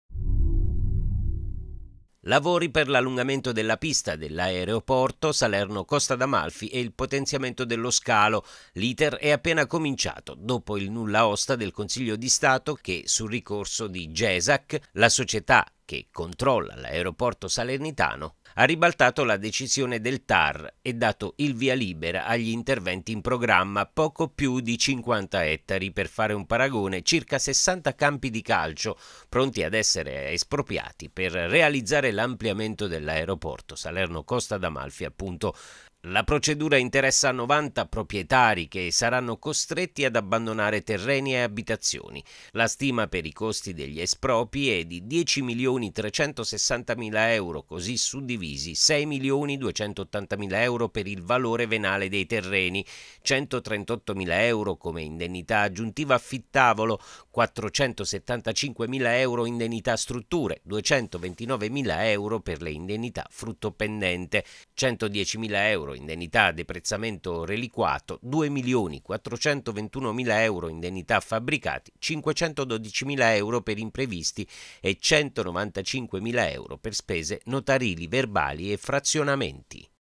Salerno, al via l'iter per il potenziamento dell'areoporto
Lavori per l’allungamento della pista dell'aereoporto salerno costa d'amalfi e il potenziamento dello scalo, iter è appena cominciato, dopo il nulla osta del Consiglio di Stato che, su ricorso di Gesac (la società che controlla l’aeroporto salernitano guidata dal presidente Carlo Borgomeo e dall’Ad Roberto Barbieri ) - ha ribaltato la decisione del Tar e dato il via libera agli interventi in programma.